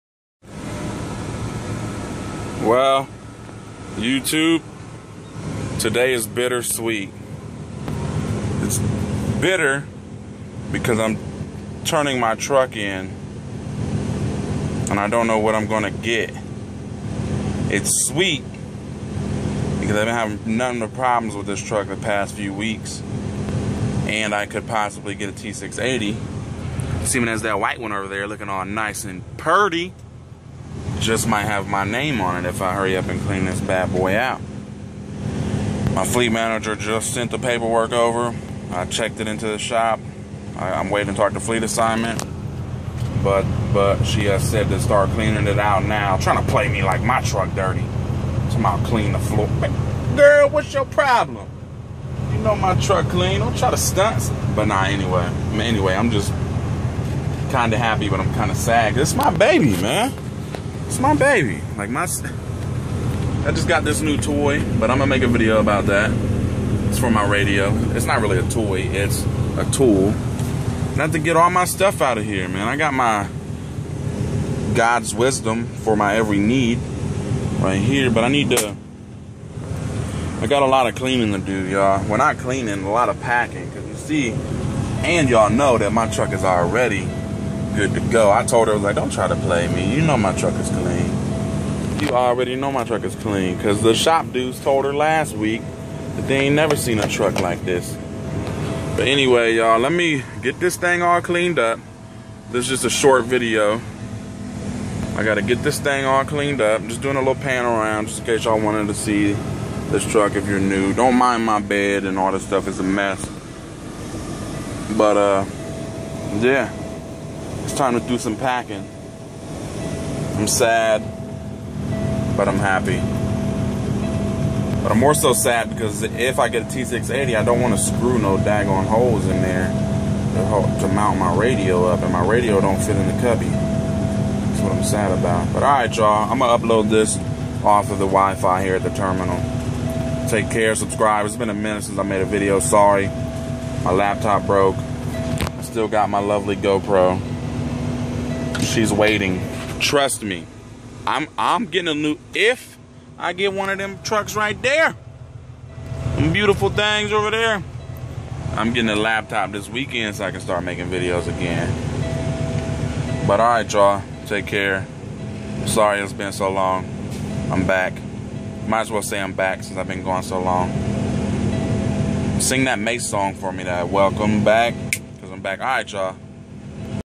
Truck swap
Hopefully I get a brand new T680 so I don't have to deal with problems on a old truck